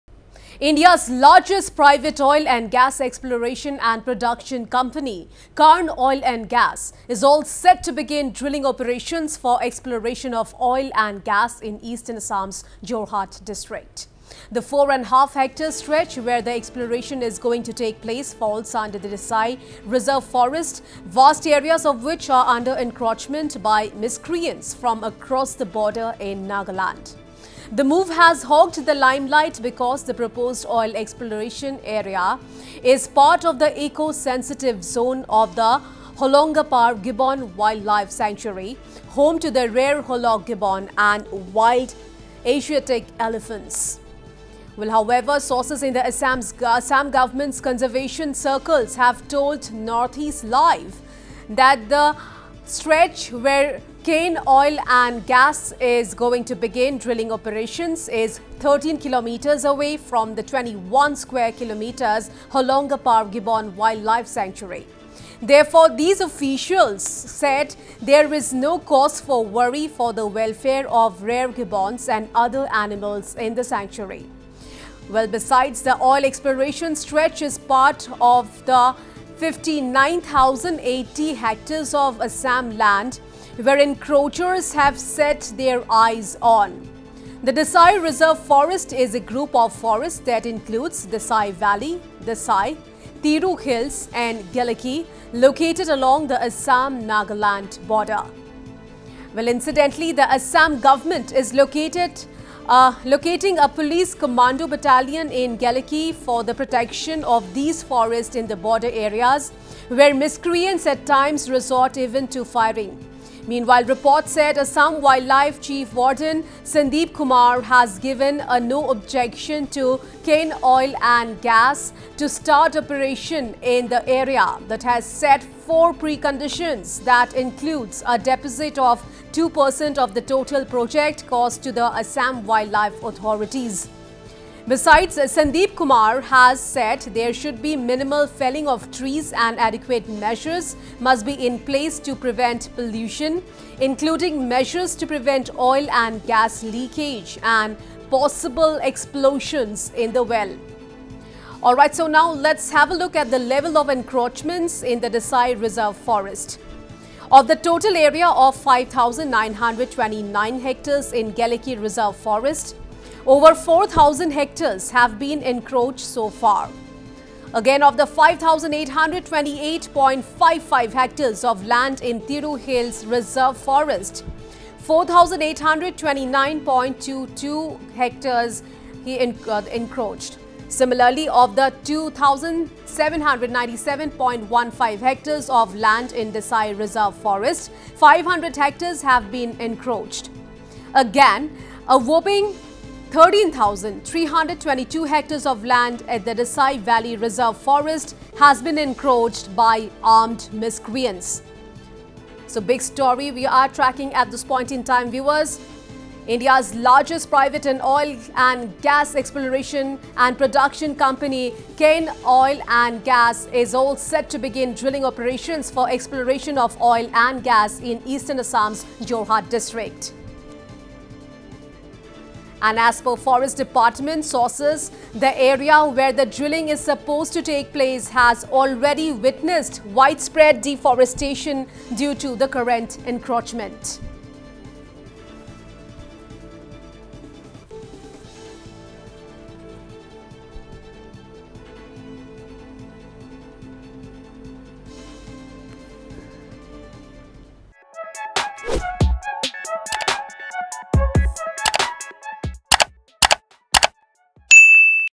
Cairn Oil & Gas's Drilling Ops In Dissai Reserve Forest
Cairn Oil And Gas's Drilling Ops In Dissai Reserve Forest Not to Hurt Holongapar Gibbon Wildlife Sanctuary

India’a largest private oil and gas exploration and production company, Cairn Oil & Gas, is all set to begin drilling operations for exploration of oil and gas in eastern Assam’s Jorhat district.
The four and half hectare stretch where the exploration is going to take place falls under the Dissai Reserve Forest, vast areas of which are under encroachment by miscreants from across the border in Nagaland.
The move has hogged the limelight because the proposed oil exploration area is part of the eco-sensitive zone of the Holongapar Gibbon Wildlife Sanctuary, home to the rare Hollock Gibbon and wild Asiatic elephants.

However, sources in the Assam Government’s conservation circles have told Northeast Live that the stretch where Cairn oil and gas is going to begin drilling operations is 13 kms away from the 21 square kilometre Holongapar Gibbon Wildlife Sanctuary.